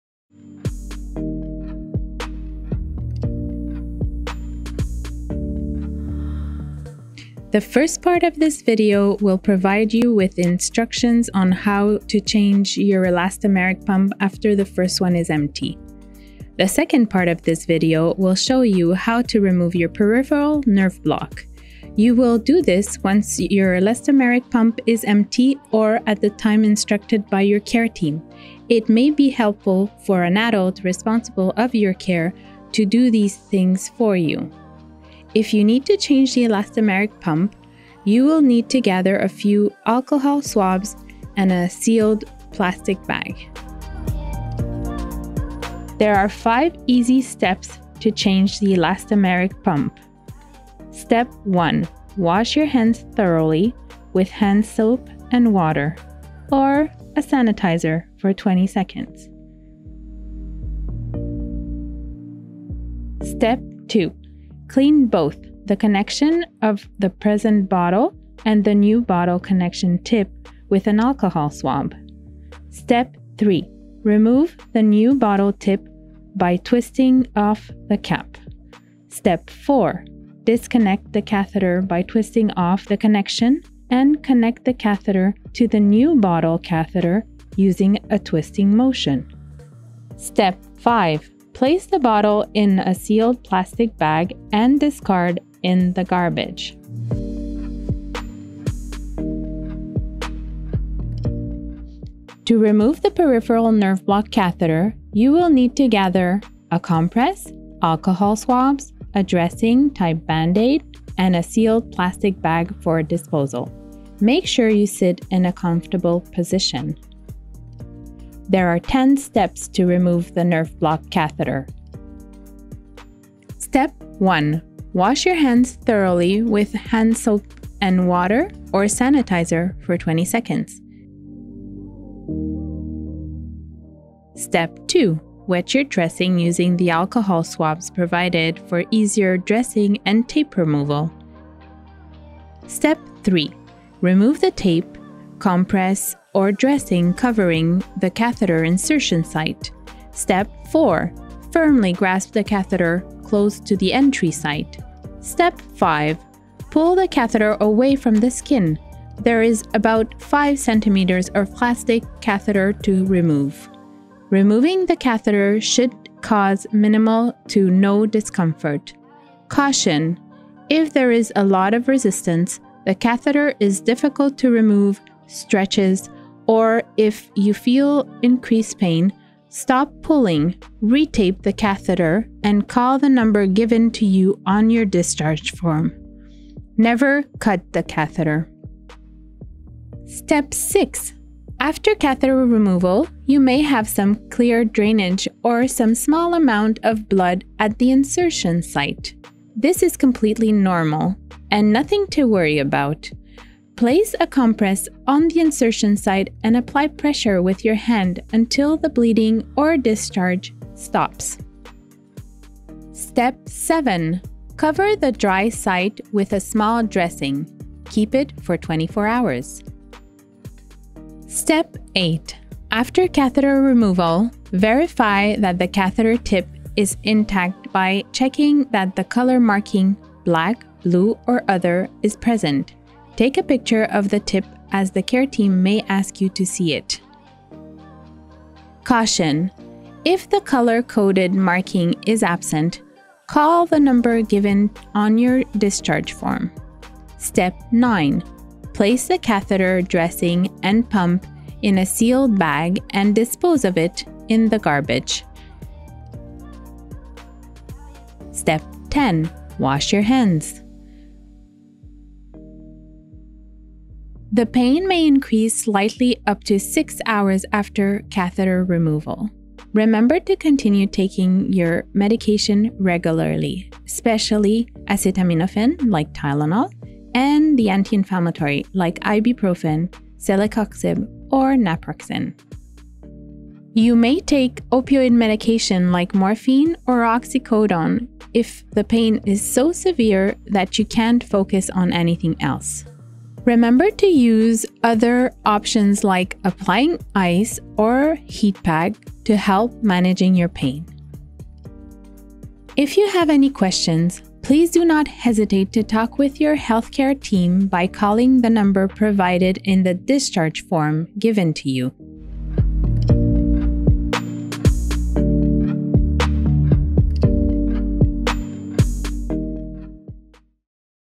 Elastomeric Pump and Catheter Removal
How to remove an  Elastomeric Pump and Catheter at home.